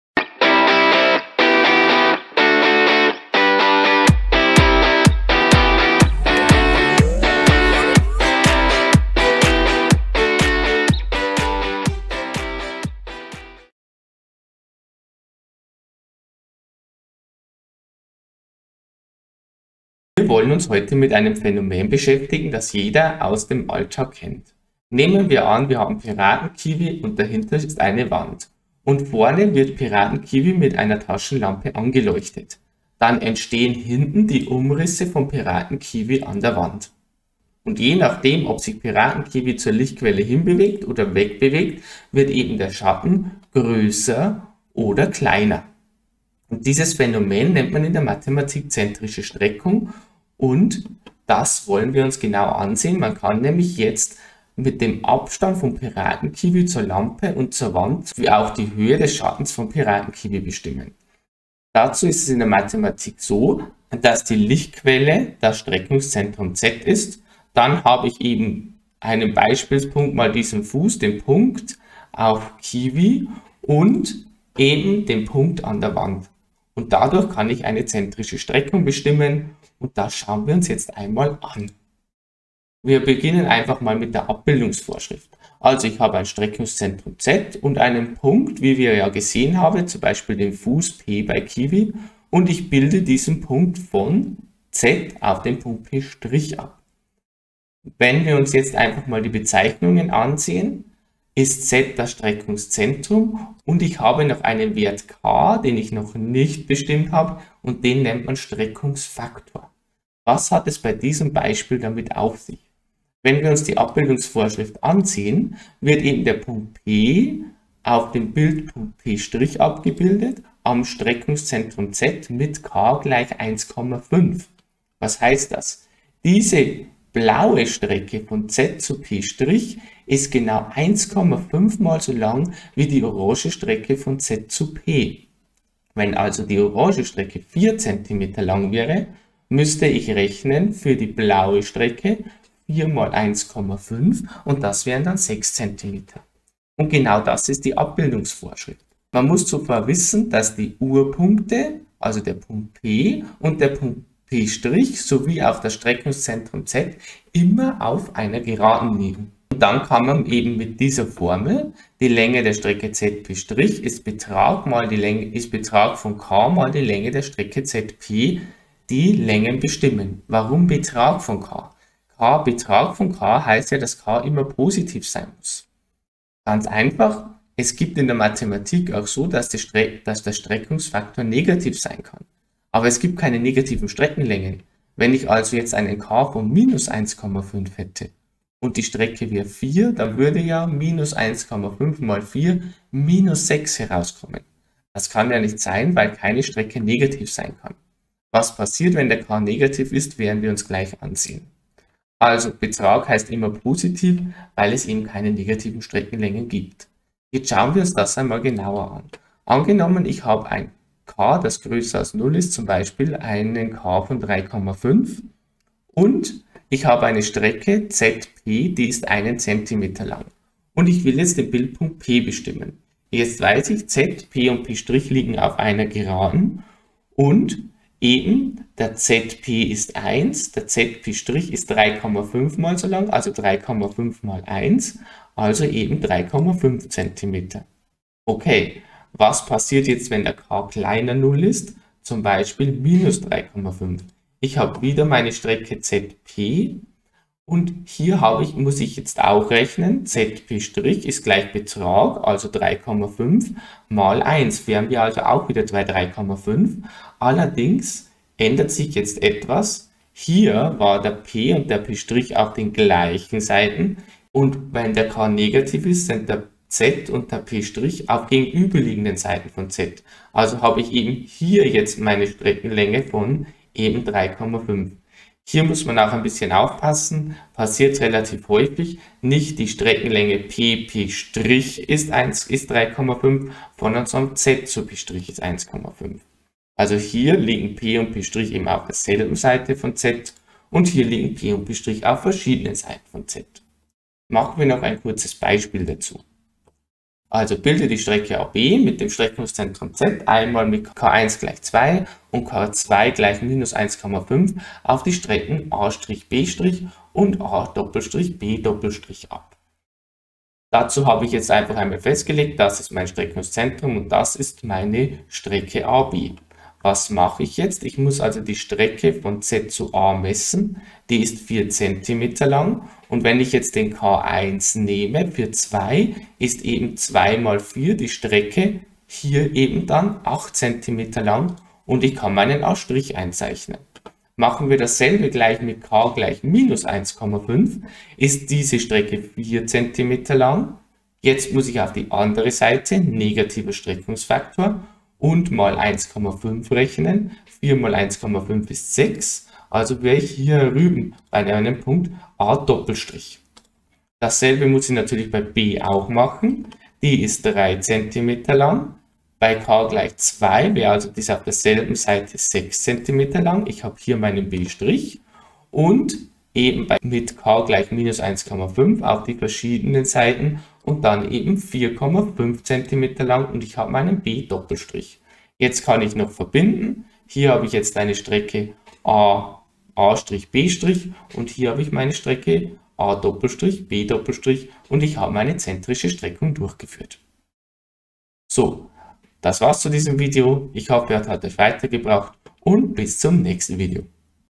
Zentrische Streckung Einführung und Abbildungsvorschrift
In diesem Video beginnen wir mit dem Thema der zentrischen Streckung.
Was ist eine zentrische Streckung?
Wie wird sie durchgeführt?
Wie lautet die Abbildungsvorschrift?

0:00 - Intro
0:20 – Einführung: Zentrische Streckung im Alltag
1:21 – Erklärung und Abbildungsvorschrift
4:55 – Beispiel